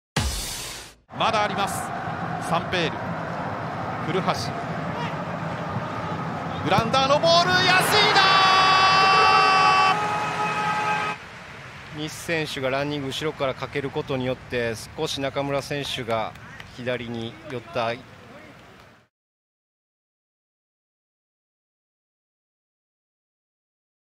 【GOAL】安井 拓也（24'）｜2020明治安田生命Ｊ１リーグ 第16節「ヴィッセル神戸vs.FC東京」
ショートカウンターから抜け出した古橋が鋭いシュートを放つが、右ポストを直撃。跳ね返りを拾ったサンペールからPA右の古橋につなぎ、古橋が折り返したボールをフリーで走り込んだ安井がきっちり決めてゴール！安井のJ1リーグ初得点で神戸が先制！

［こちらから新規入会でクラブへ強化費還元！］